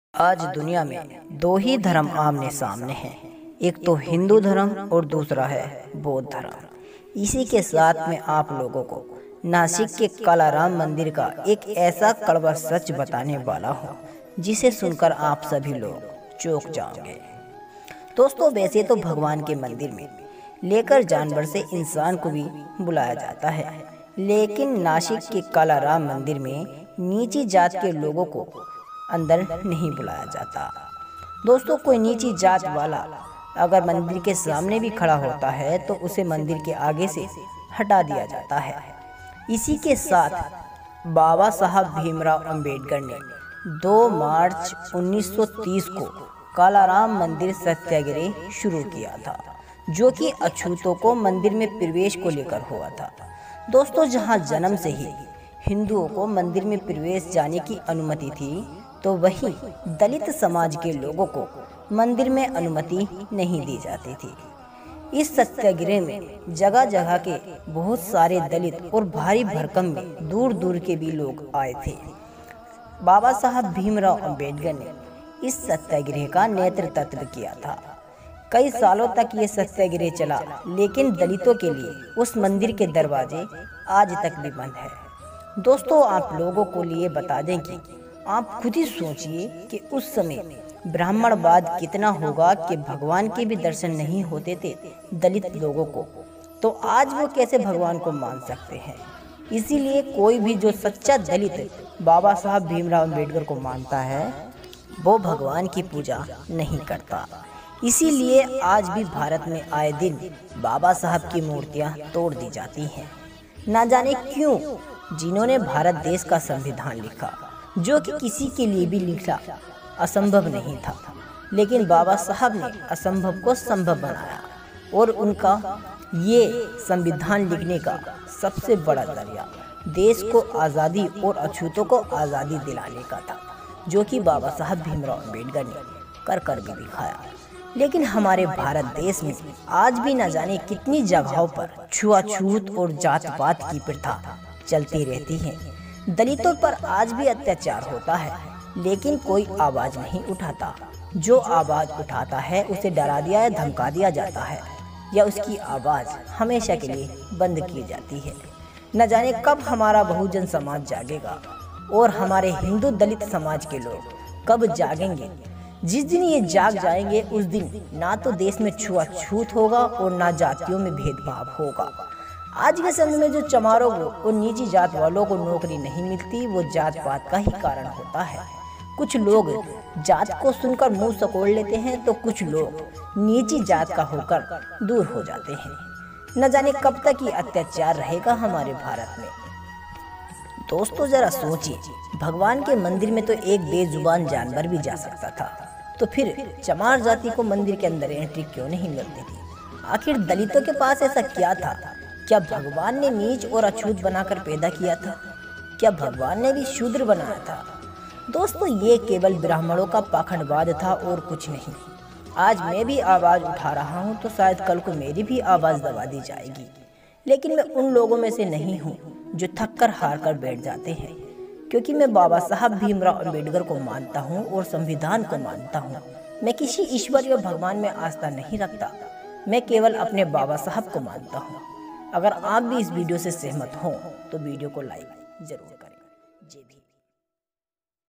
चमार जाति के लोग इस वीडियो को जरूर देखें | full history of Dr br Ambedkar
दोस्तों हमारा भारत इसी लिए आज आशा है क्योंकि यहां जाती बाद करने वाले आज भी मौजूद है